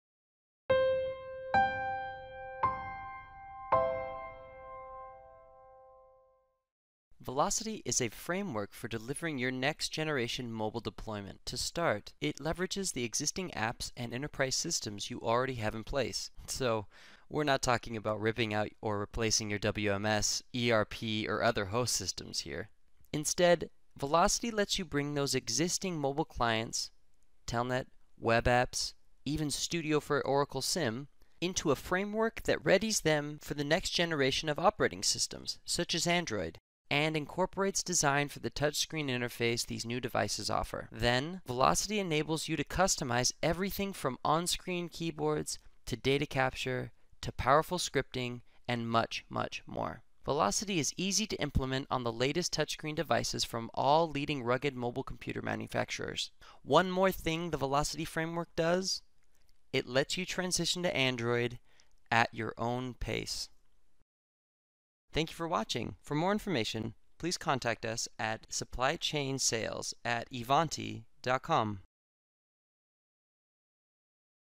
Velocity – Next Generation Supply Chain Mobility
Next Generation Supply Chain Mobility-

The Velocity framework combined with the M.O.M.S. platform will allow your company to migrate successfully into the future of mobility. Don’t worry! Our Ivanti Supply Chain team has you covered!